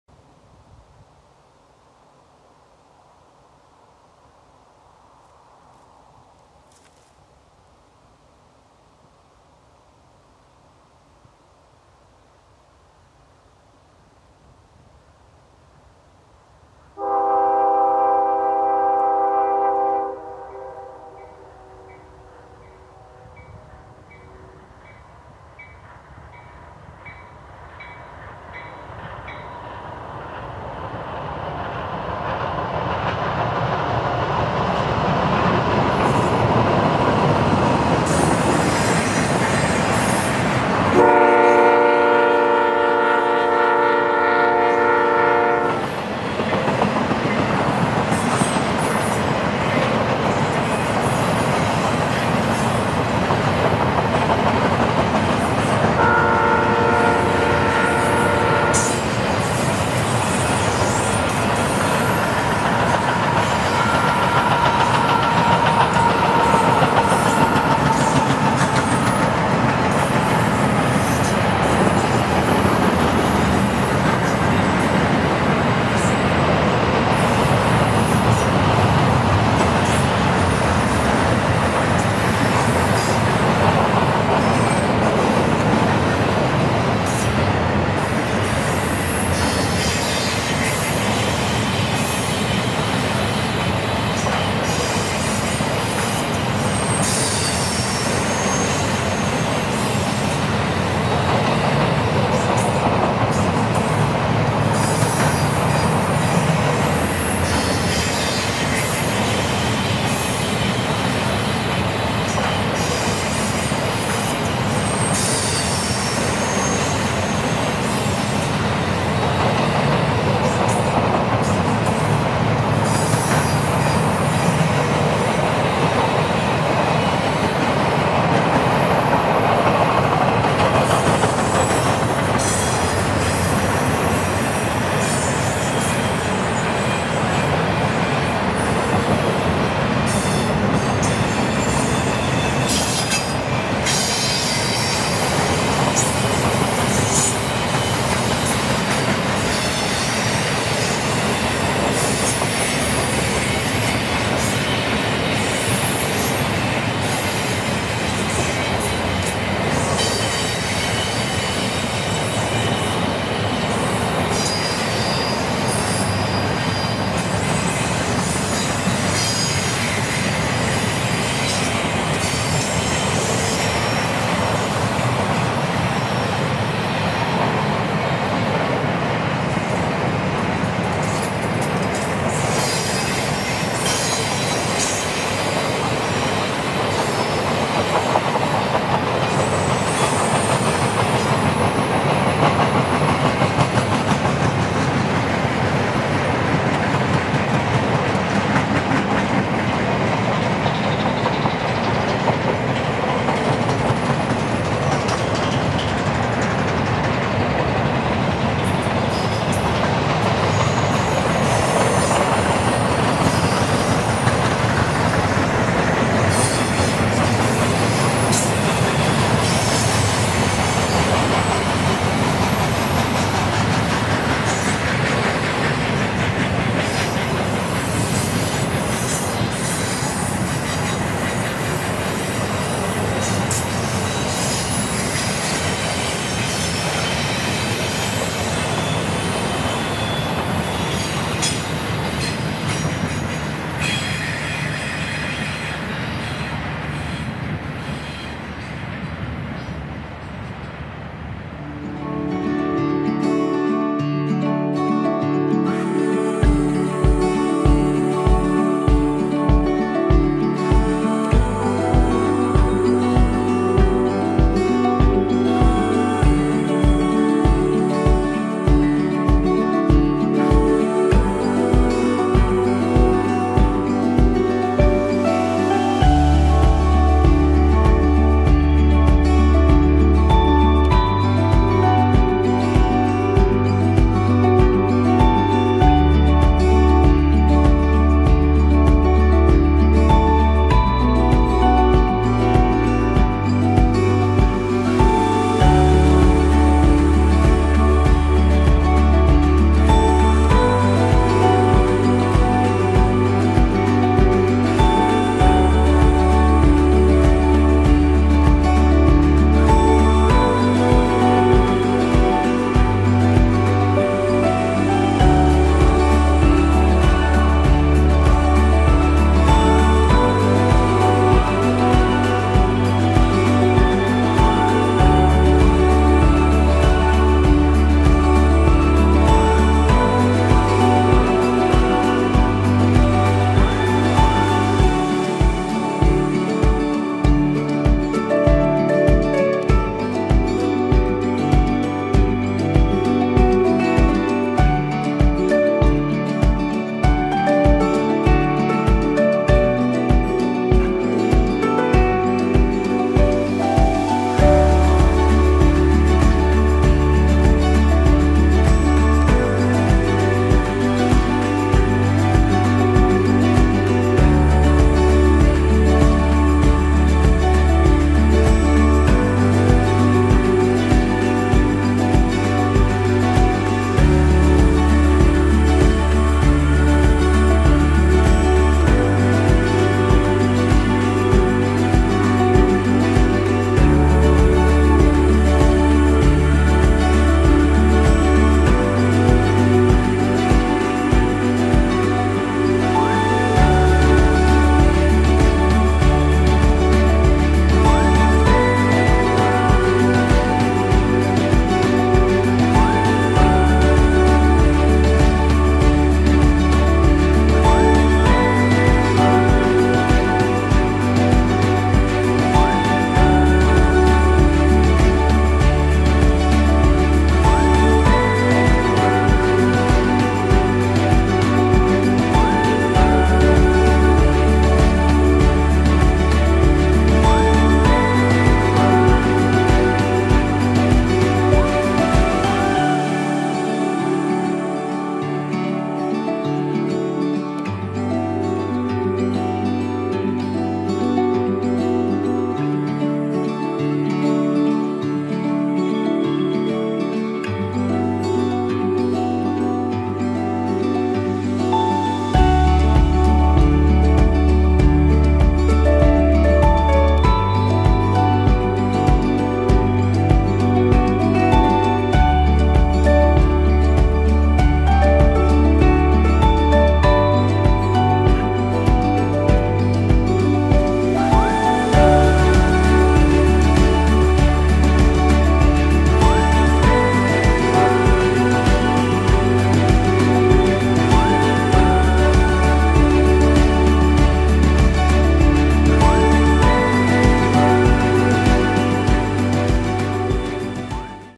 CSX Intermodal I028 with CSX 1776 in mid train DPU
I028 rolls north near Vincennes, IN with the 1776 unit as a mid train DPU.